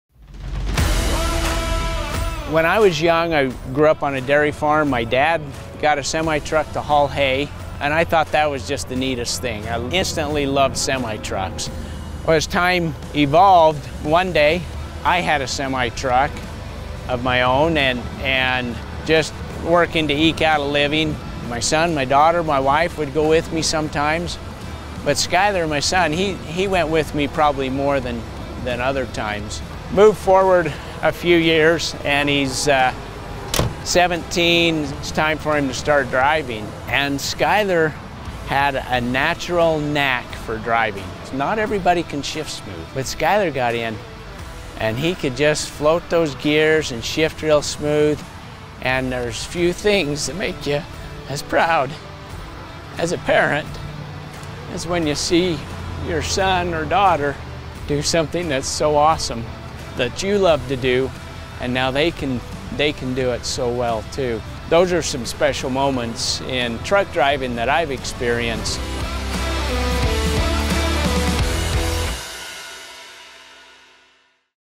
Pride Through The Generations | Cat® On-Highway Truck Engines
When his father bought a semi truck for the family dairy farm, it sparked a lifelong passion — and countless stories from the road — for Mark Droubay of Double D Distribution. Years later, that passion was passed down to Mark’s son Skyler, who has shown a natural knack for the trucking life. Learn how their shared talents and passion are points of pride for multiple generations of the Droubay family. Find more stories from the road and repair tips